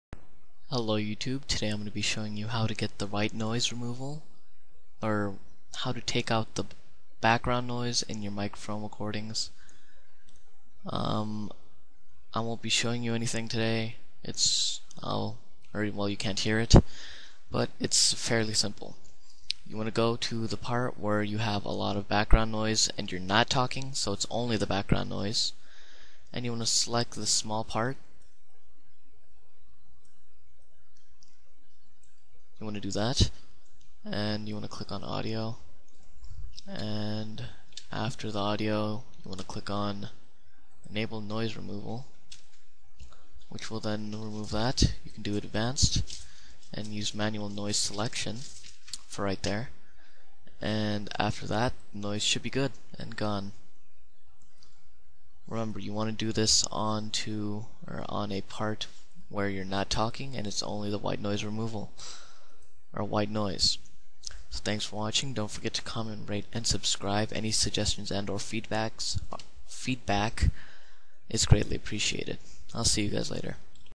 How to get rid of background noise [White Noise Removal]
I'll be showing you how to get rid of white noise with Camtasia Studio! It's fairly simple and is very quick!